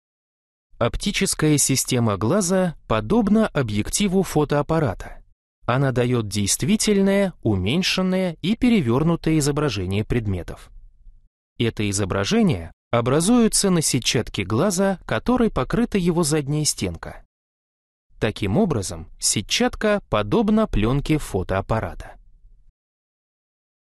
Преломление лучей в оптической системе глаза
Оптическая система глаза подобно объектива фотоаппарата.
Она дает действительное, уменьшенное и перевернутое изображение предметов.
Это изображение образуются на сетчатке глаза, который покрыта его задняя стенка.
Таким образом сетчатка подобно пленки фотоаппарата.

За учебником Физика 7 класс Перышкин А. В.